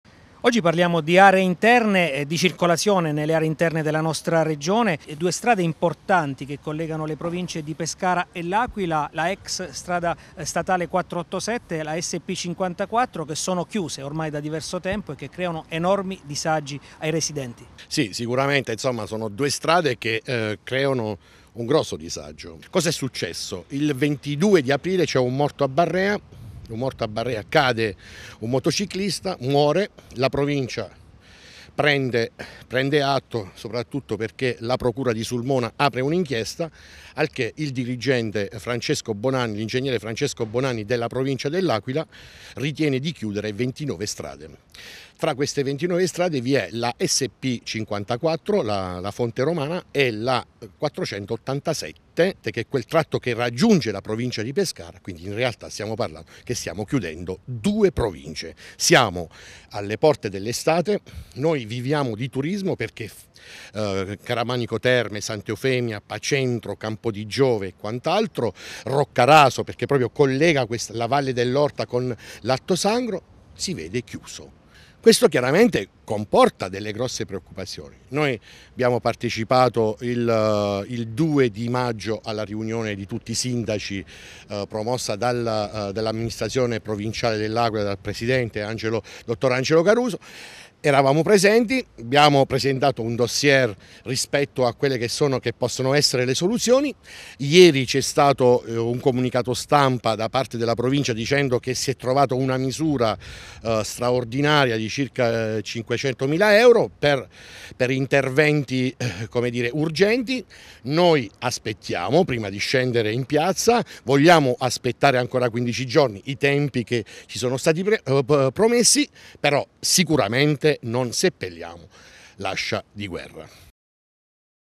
Viabilità compromessa nel Parco della Maiella
Le strade Ex SS 487 e Sp 54 chiuse in provincia di Pescara, stanno creado disagi ai residenti, con collegamenti sempre più complicati con la provincia dell'Aquila. La denuncia arriva dal presidente dell'associazione Terre Maiella-Morrone Antonello Di Giovine